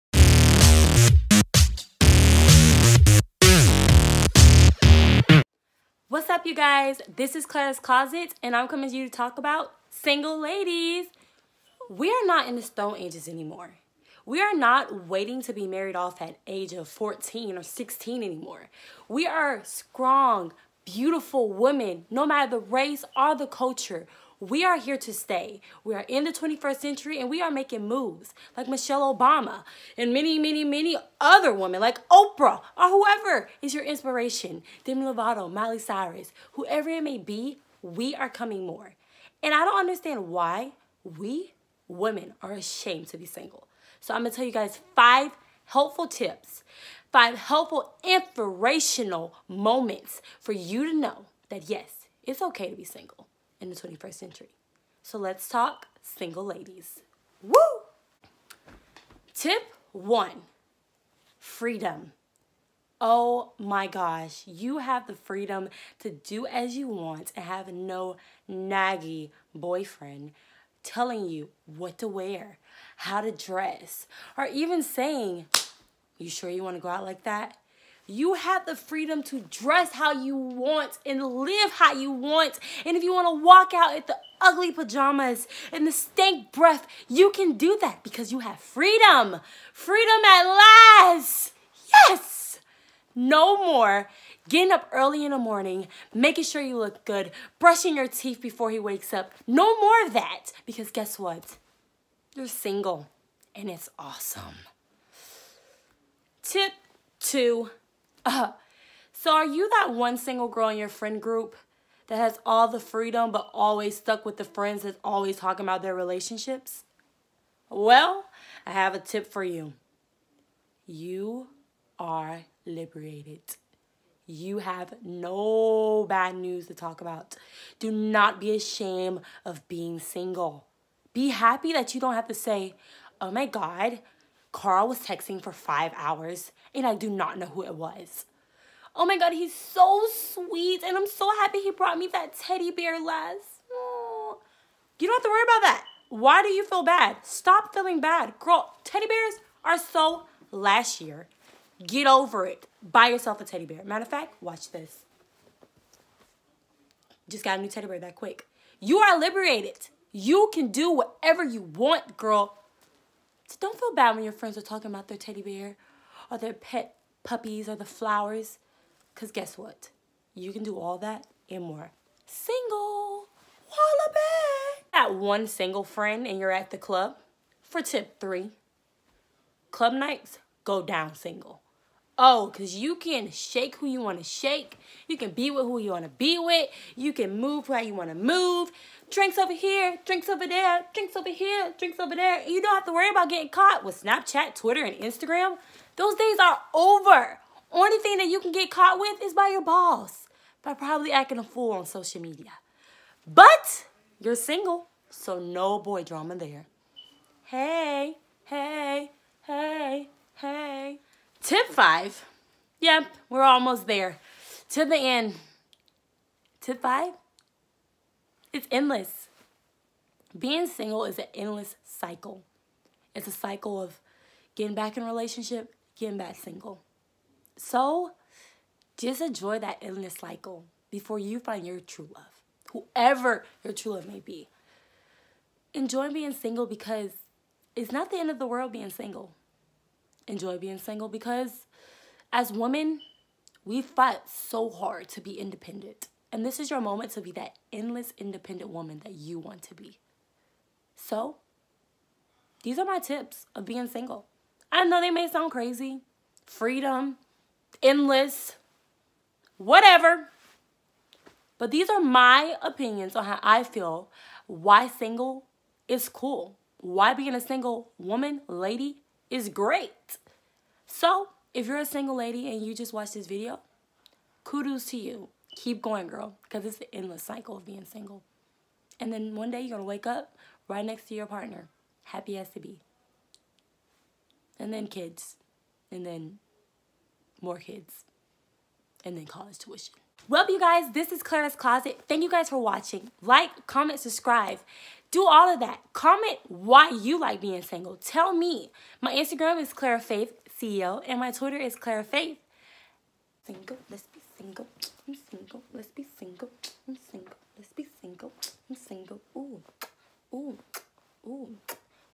Let’s Talk : Single Ladies
Let’s Take : Single Ladies, gives all my lovely Single Ladies five GREAT reason why you should be happy be single!